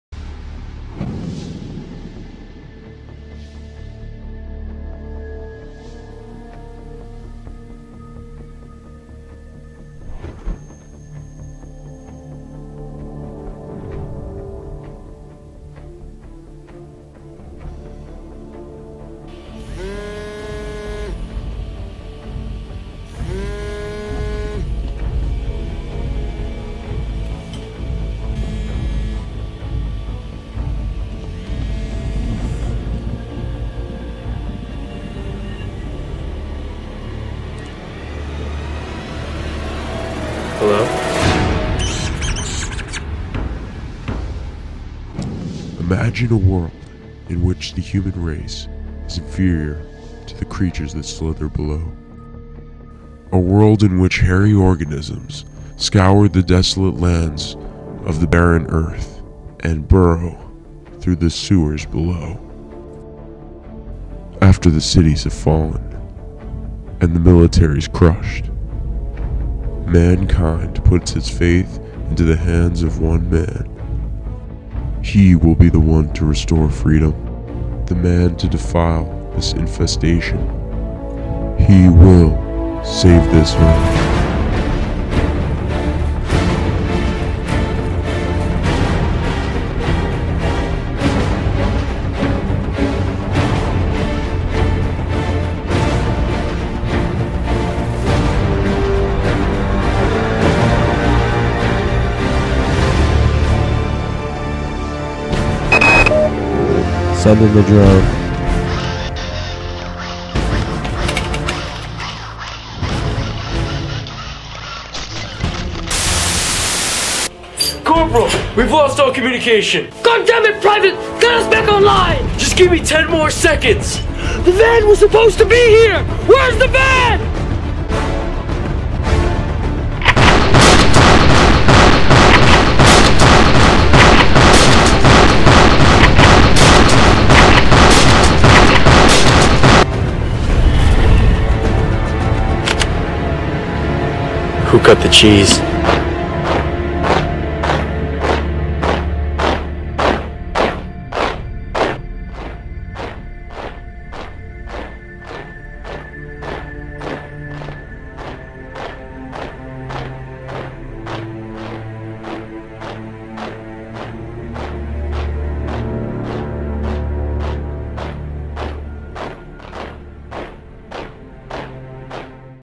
The Exterminator Official Trailer
A fake movie trailer for a Media Advertising project.